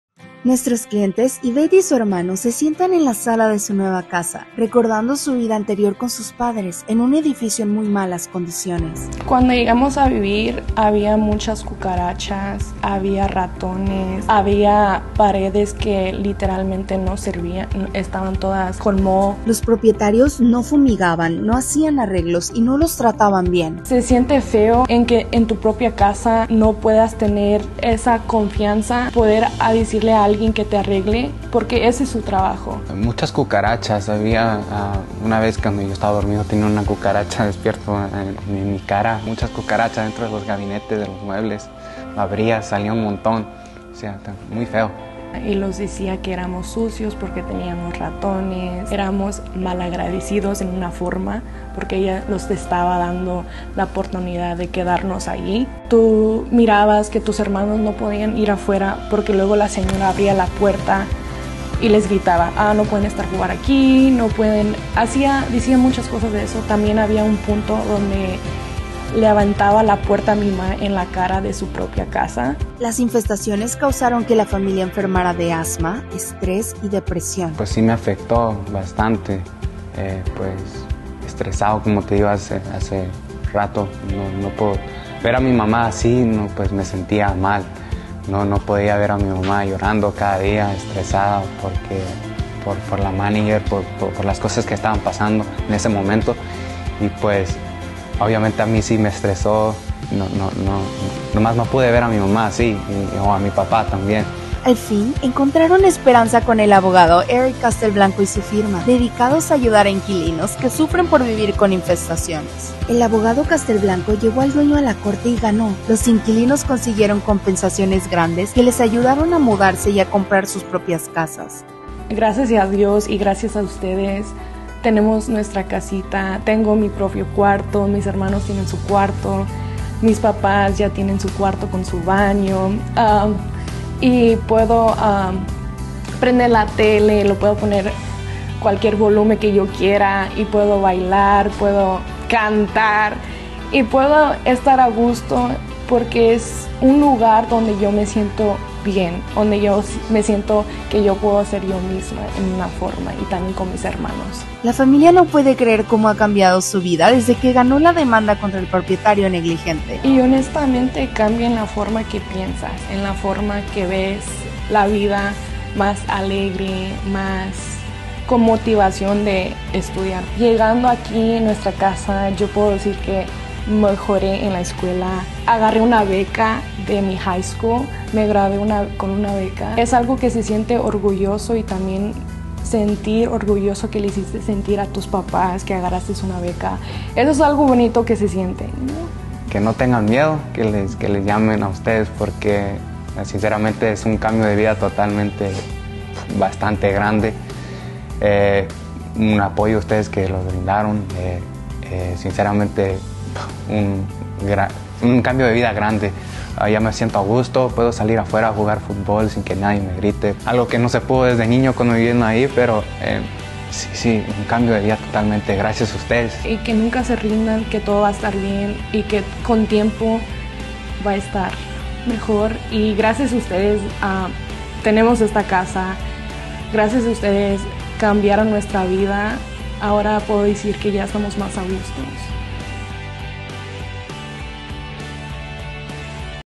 Casos de Éxito: Inquilinos Cambiaron Su Vida De Vivir En Malas Condiciones a Comprar Su Propia Casa🏡
Yvette y Alejandro nos comparten su historia, viviendo en un apartamento en malas condiciones y soportando malos tratos por un arrendador negligente.
Castelblanco Law Group ayuda a inquilinos como Yvette y Alejandro a obtener compensación por su dolor y trauma.🏡
📞Llama hoy para una revisión de caso gratis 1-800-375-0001

Facebook: Castelblanco Law Group, APLC